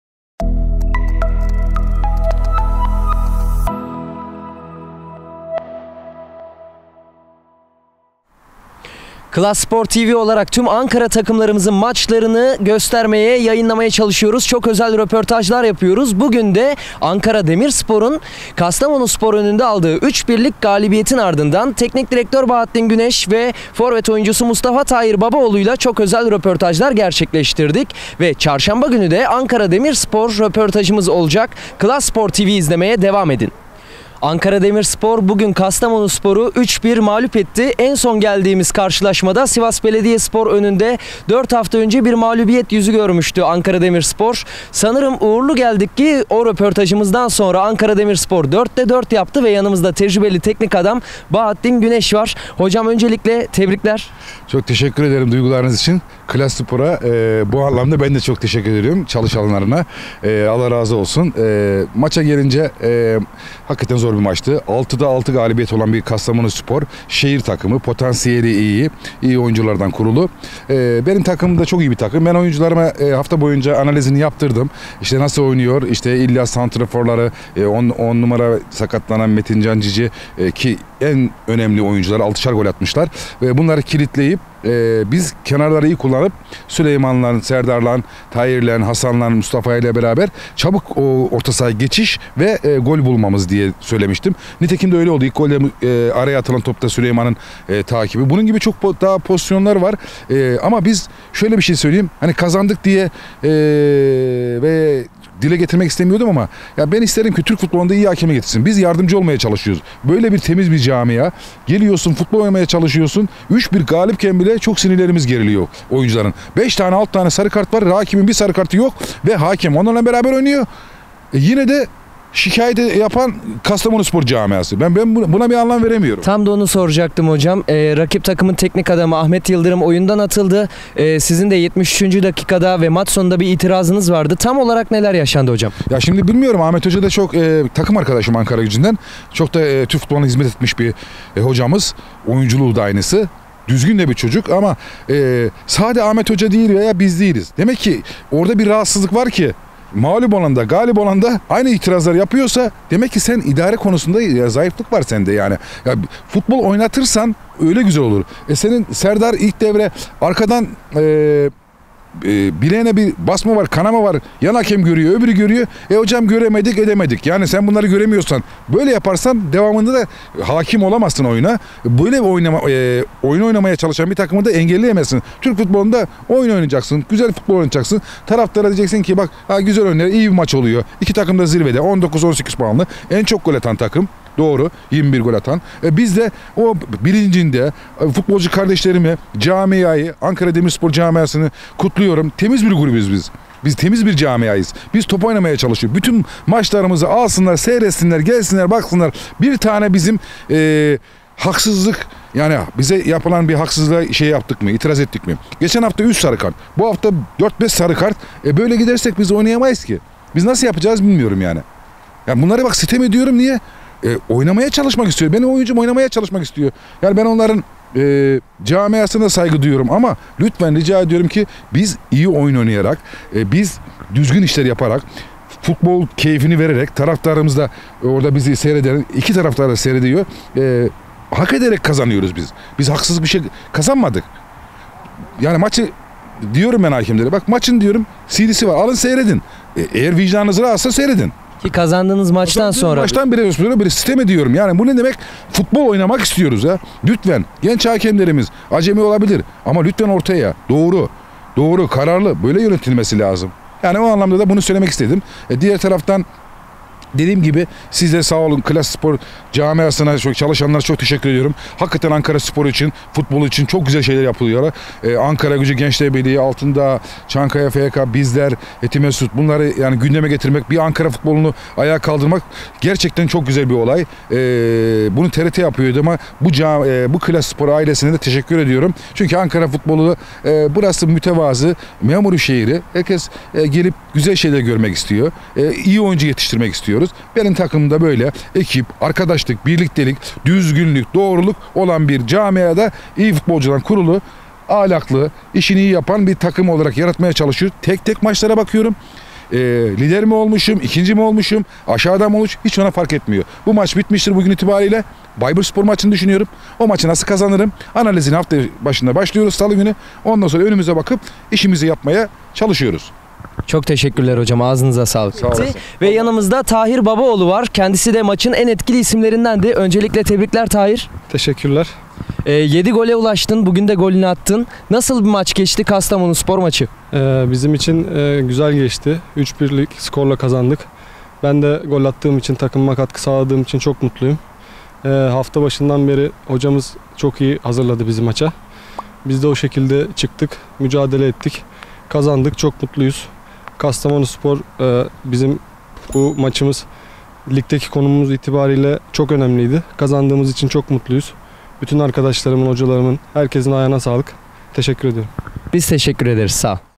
ANKARA DEMİRSPOR 4'DE 4 YAPTI
TFF 2.lig kırmızı grupta 10. hafta mücadelesinde Ankara Demirspor, rakibi Kastamonuspor'u 3-1 mağlup ederek ligde son 4 maçını da kazanmış oldu.